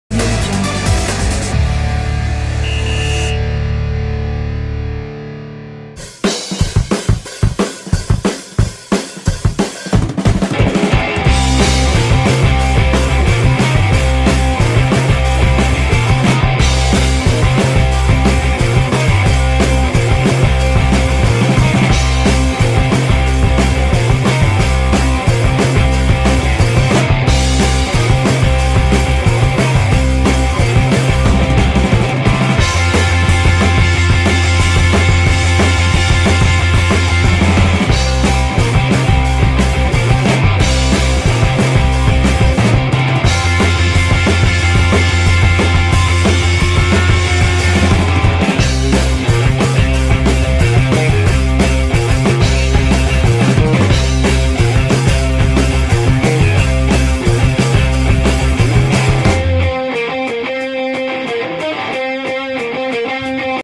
ZLATAN IBRAHIMOVIC SPECTACULAR SCORPION GOAL VS TORONTO FC
Incredible Scorpion Goal of zlatan ibrahimovic, Galaxy vs Toronto FC 3-3 MLS